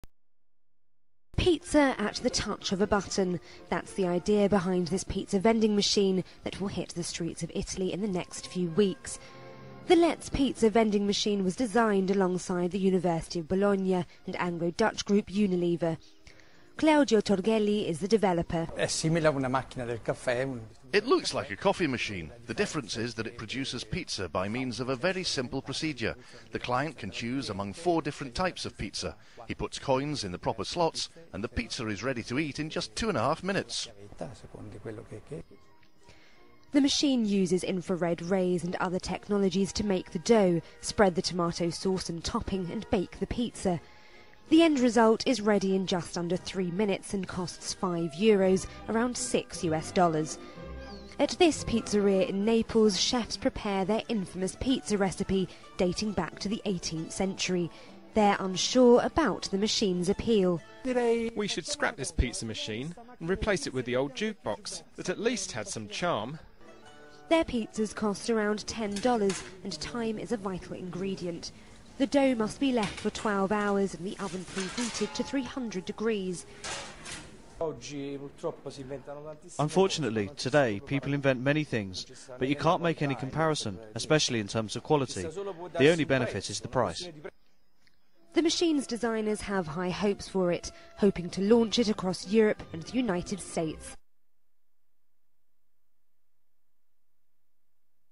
expendedor de pizza Italia y su tegnología
Este invento, con tecnología desarrollada en la universidad de Bolonia, ha sido bautizado como el 'Let's Pizza'. Usa rayos infrarrojos para amasar la harina y mezclarla con agua, ponerle salsa de tomate y una selección de relleno.

No olviden inscribirsen, opinar, y visitarnos en nuestra página...día a día con la actualidad